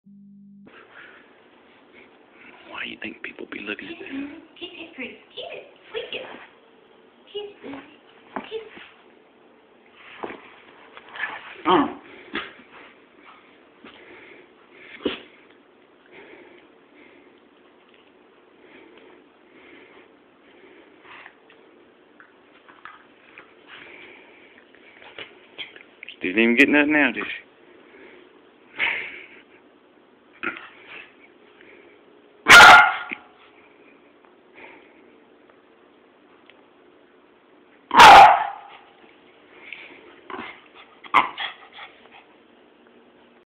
Silly Lily the funny westie dog
Lily playing and digging for toy